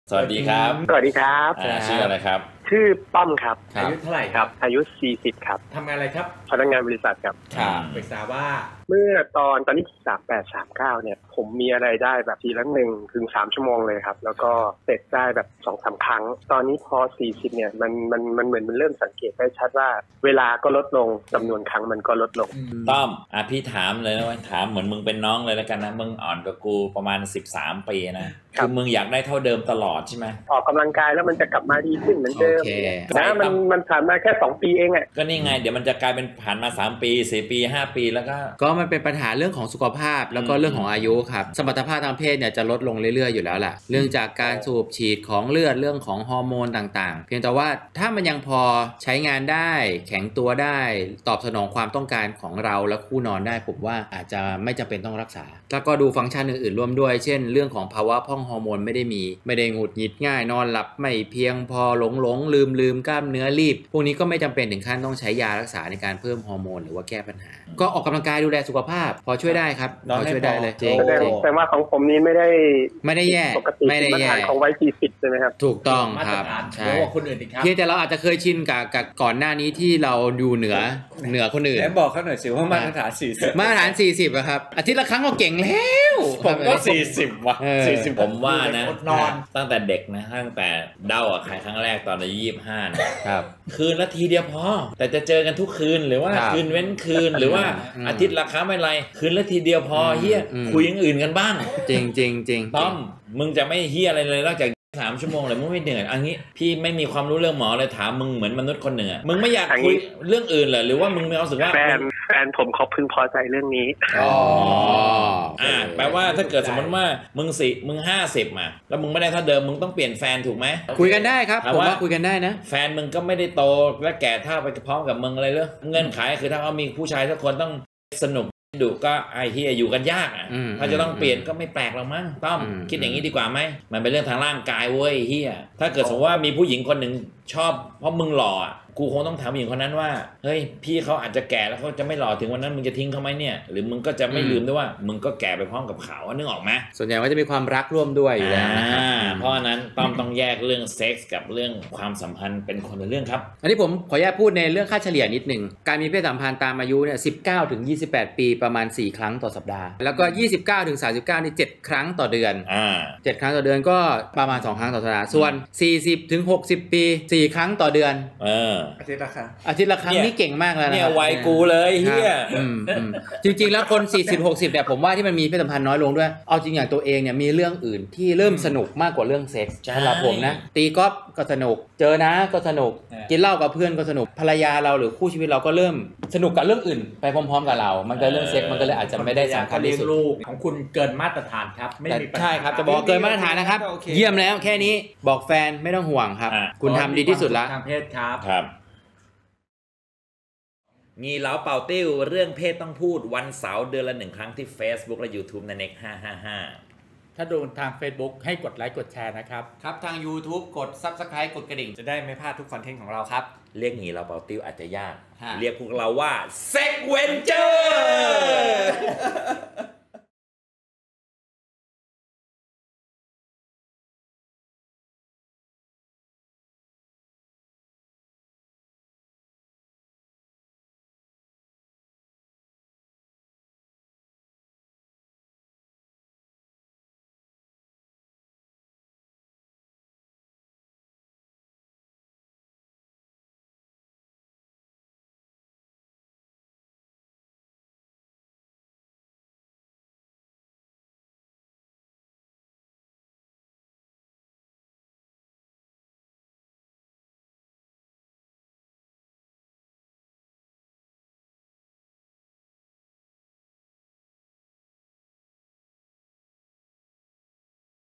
อายุมากขึ้น...สมรรถภาพทางเพศลดลง?!?!
ที่เขาว่ากันว่าพออายุมากขึ้นแล้วกระสุนจะค่อยๆลดลง เป็นเรื่องจริงหรือเปล่า?!?!?!

คู่ขนาน 2 ช่องทาง เฟสบุ๊ค-ยูทูป